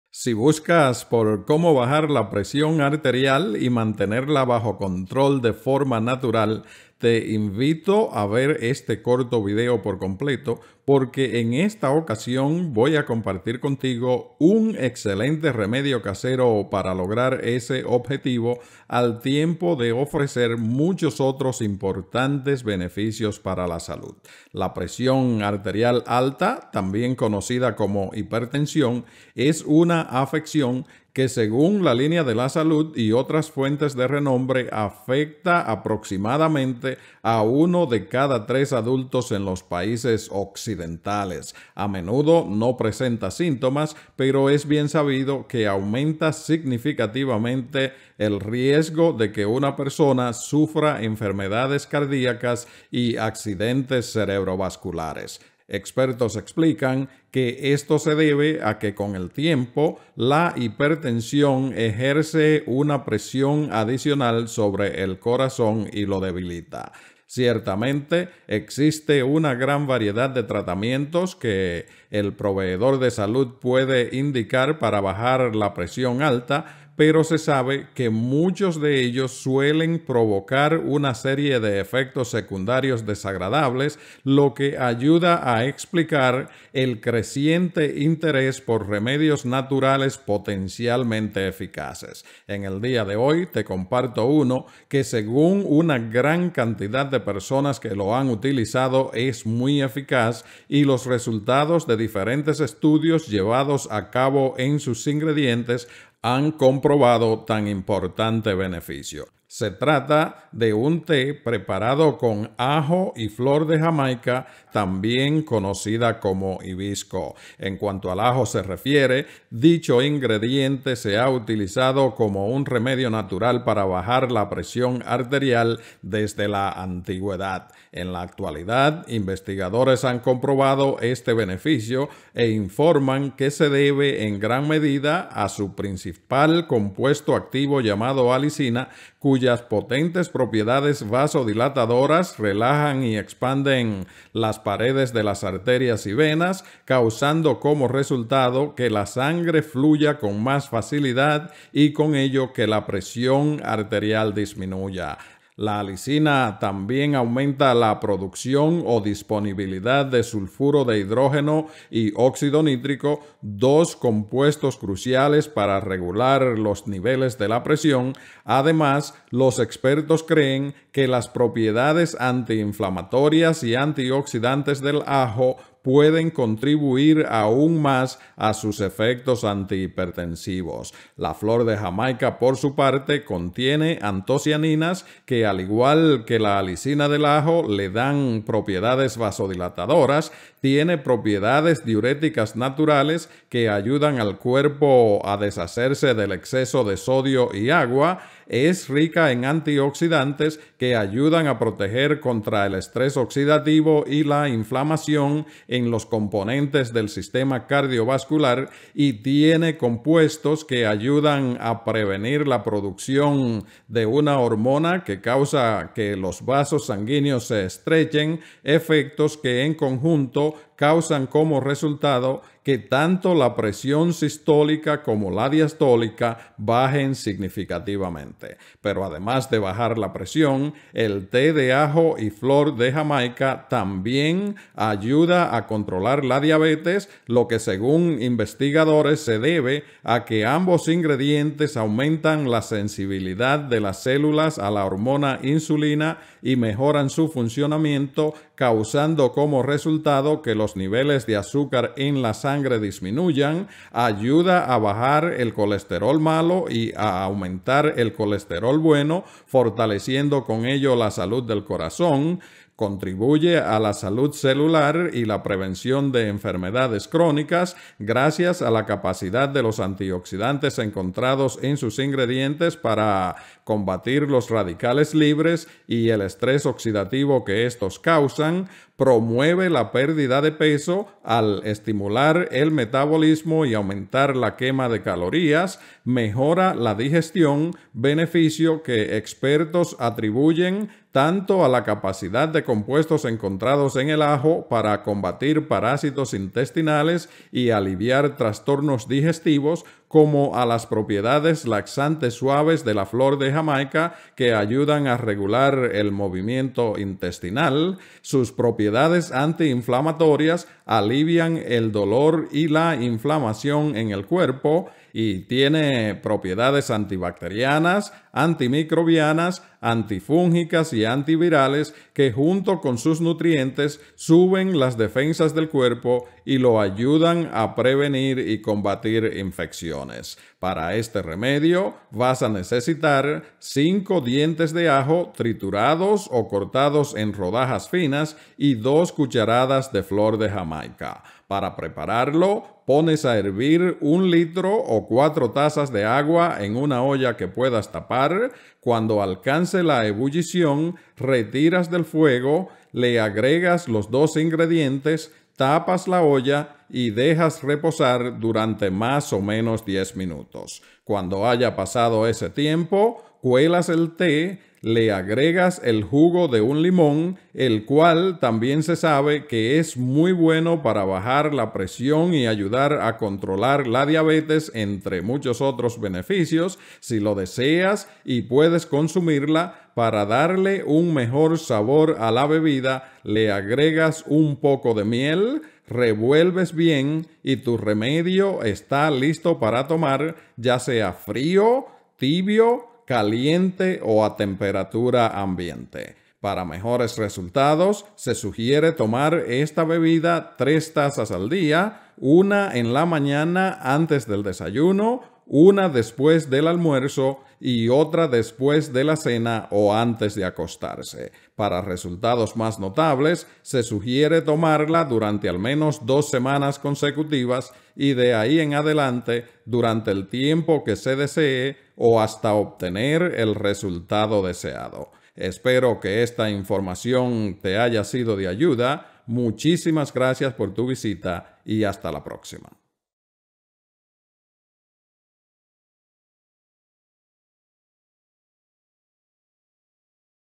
ESTO BAJA LA PRESION ARTERIAL COMO POR ARTE DE MAGIA También Baja El Azúcar y Colesterol
Si buscas por como bajar la presión arterial y mantenerla bajo control de forma natural te invito a ver este corto video por completo, porque en esta ocasión voy a compartir contigo un excelente remedio casero para bajar la presión eficazmente, al tiempo de ofrecer muchos otros importantes beneficios para la salud, incluyendo bajar el azúcar en la sangre y el colesterol malo, entre muchos otros

La presión arterial alta, también conocida como hipertensión, es una afección que, según la línea de la salud, afecta aproximadamente a uno de cada tres adultos en los países occidentales. A menudo no presenta síntomas, pero es bien sabido que aumenta significativamente el riesgo de que una persona sufra enfermedades cardíacas y accidentes cerebrovasculares

Expertos explican que esto se debe a que, con el tiempo, la hipertensión ejerce una presión adicional sobre el corazón y lo debilita.

Ciertamente, existe una gran variedad de tratamientos que el proveedor de salud puede indicar para bajar la presión alta, pero se sabe que muchos de ellos suelen provocar una serie de efectos secundarios desagradables, lo que ayuda a explicar el creciente interés por remedios naturales potencialmente eficaces.

Según una gran cantidad de personas que lo han utilizado, el que hoy voy a compartir contigo es muy efectivo, y los resultados de diferentes estudios llevados a cabo en sus ingredientes, han comprobado tan importante beneficio.